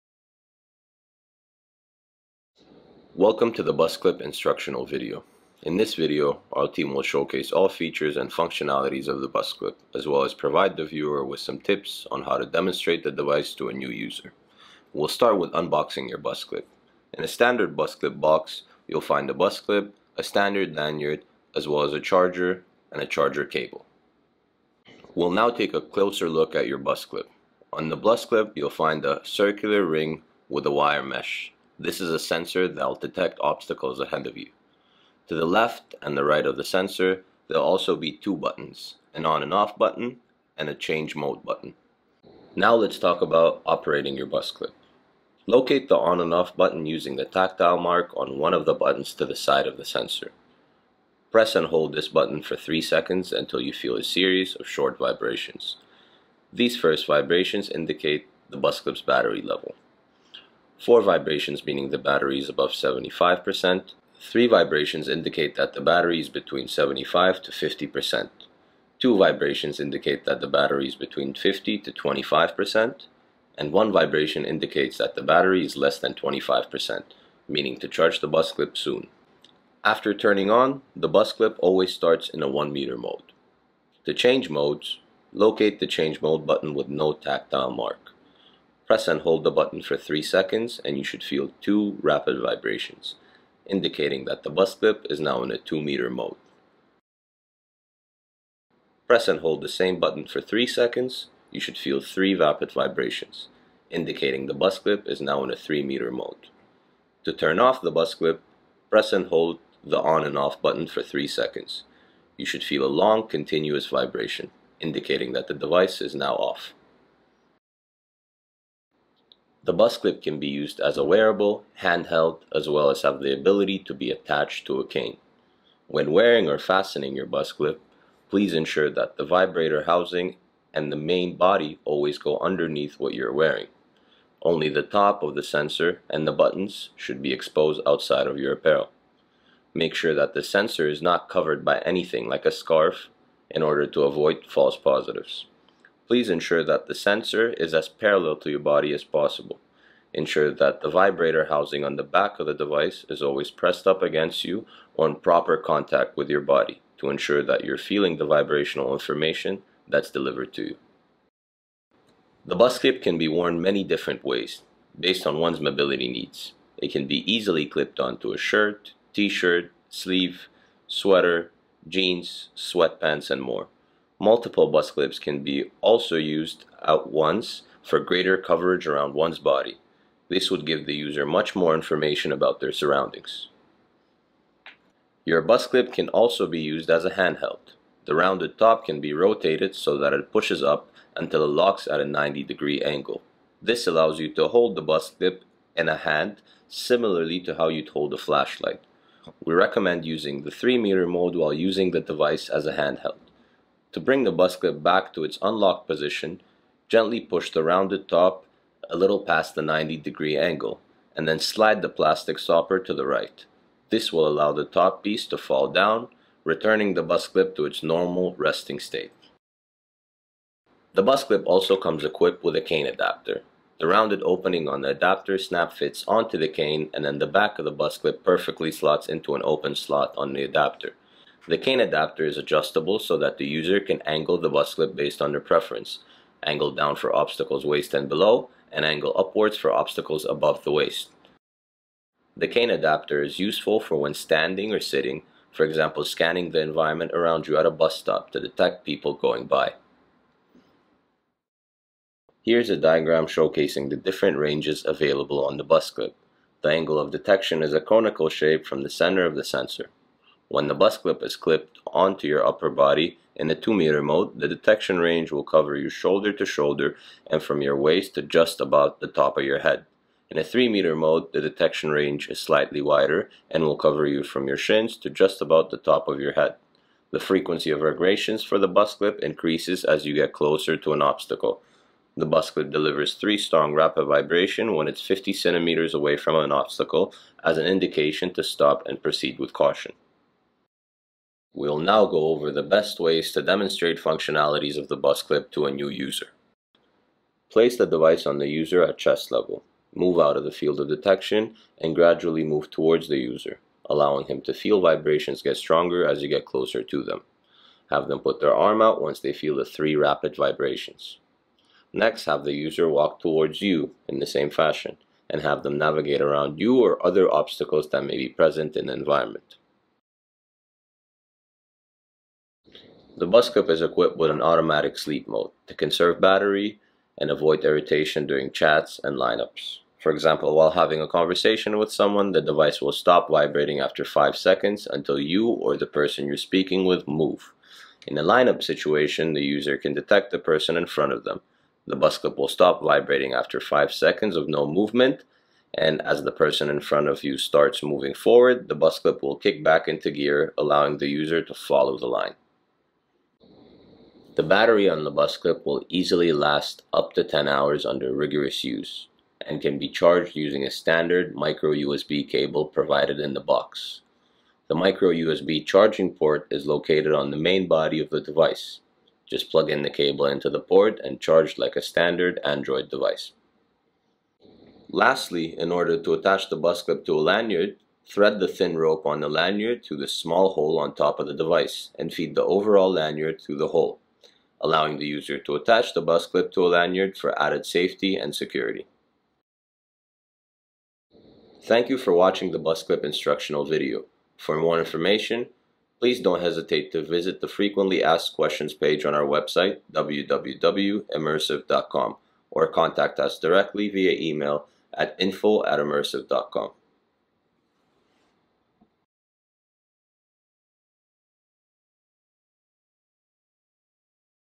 BuzzClip - Instructional Video
The is a full video manual on how to use and operate the BuzzClip, a wearable mobility tool for the blind. The instructional video is full of tips and tricks on how best to use the device in day to day situations for those living with vision loss all over the world.